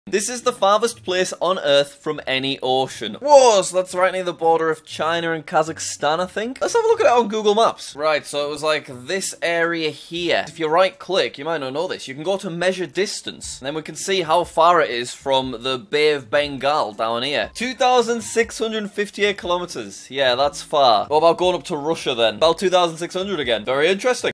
FARTHEST POINT From ANY WATER!
THE FARTHEST POINT From ANY WATER!

This point in China is the farthest point away from water in the world!